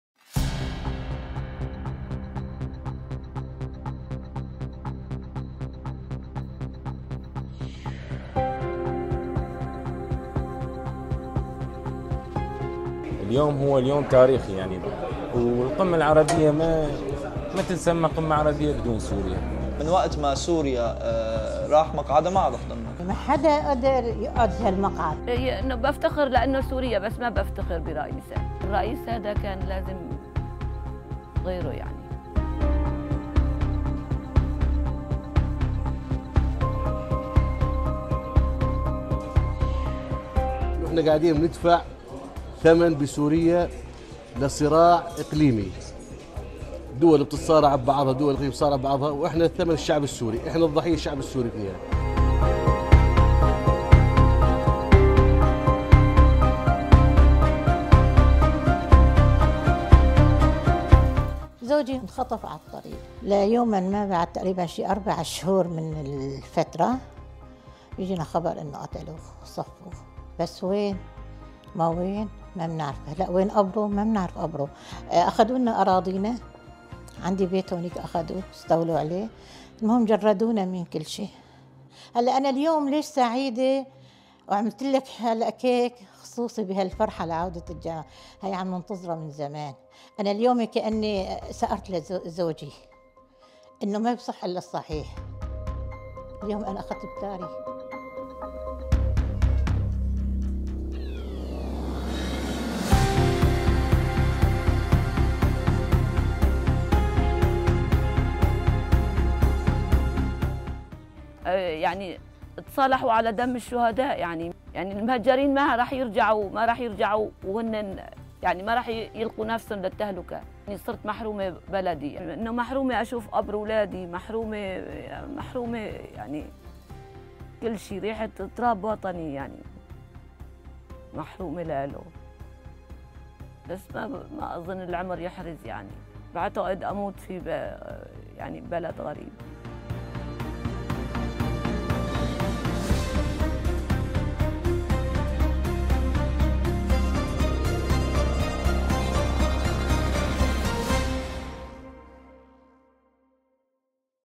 القمة العربية في عيون السوريين: أمل حذر وجراح لم تندمل
لم تكن القمة العربية في مدينة جدة هذا العام كسابقاتها بالنسبة إلى السوريين، فمشاركة الرئيس السوري بشار الأسد للمرة الأولى منذ عام 2011، قسمت الآراء بين مؤيد ومعارض لهذه الخطوة.
السوريون في دمشق وبيروت تابعوا باهتمام كلمة الأسد وكلمات الرؤساء العرب، فهل انعكست الاجواء التفاؤلية والتوافقية للقمة على السوريين داخل سوريا وخارجها؟